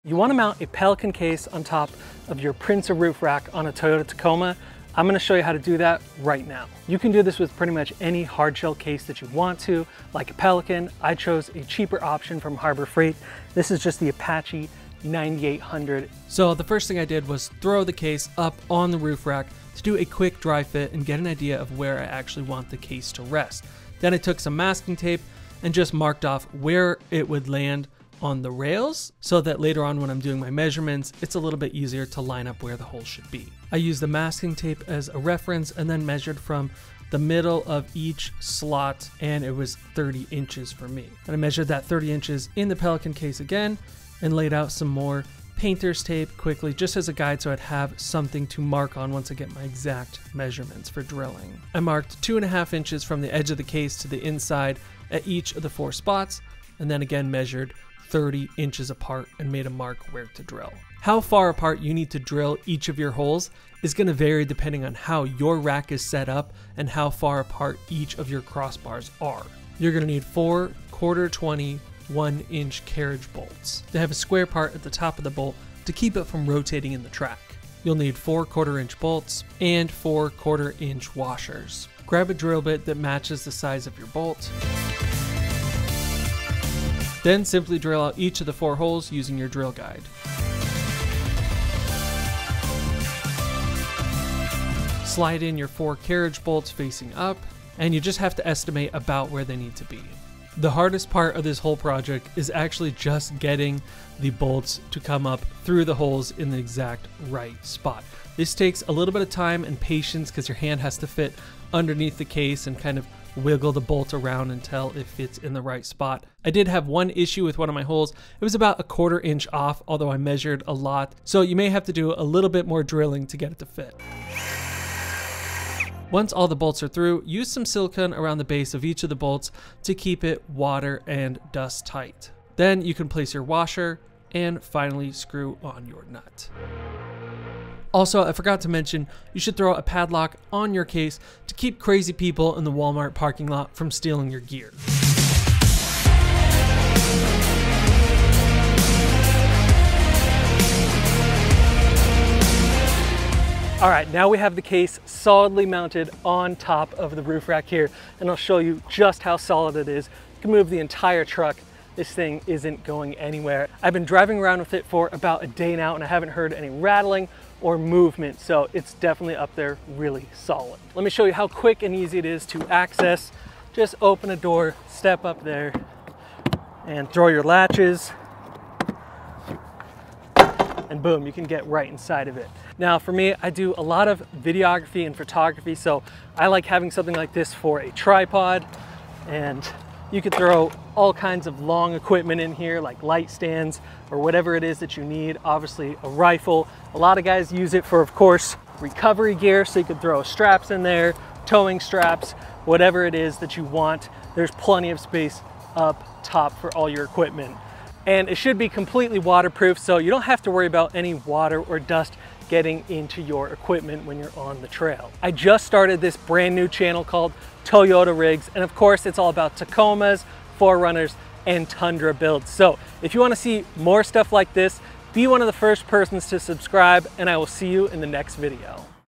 Mount a Pelican Case on a Prinsu Roof Rack on the Toyota Tacoma
A quick step-by-step guide on how to mount a Pelican case on a Prinsu roof rack on your Toyota Tacoma. Honestly, doing something similar to this will work on just about any roof rack with slotted crossbars. This is mounted on my 2014 2nd Gen Toyota Tacoma TRD Off-Road. I used the Apache 9800 Weatherproof Protective Rifle Case from Harbor Freight, since it's a cheaper option at only around $165 USD unless you find it on sale for even less!

🛠 The Gear:
Case on Amazon
Pelican 1720

Soundstripe Licenses: D5ZY1PKP00LWTRZI, FBM99MUWN11UCEAW